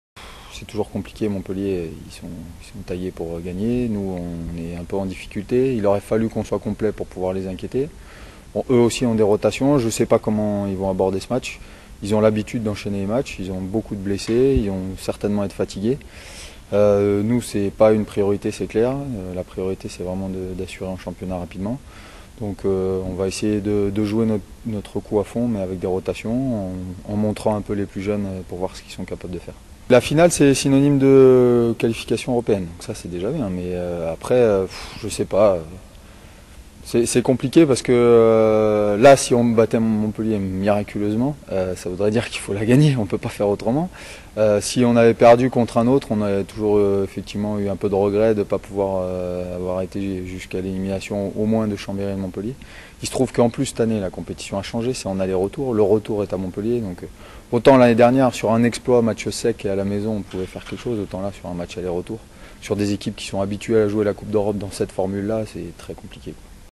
Hand : Toulouse redoute Montpellier
Enfin, on termine ce journal avec du sport et plus particulièrement du handball. Et quelques jours après une victoire cruciale à Nîmes (23/20), les Toulousains disputent une nouvelle rencontre. Cette fois-ci, il s'agit du quart de finale de la Coupe de la Ligue. Avec six joueurs blessés, la tâche s'annonce difficile face à l'ogre Montpelliérain. Raphael Geslan, entraineur du THB nous donne son sentiment à la veille de ce match de gala.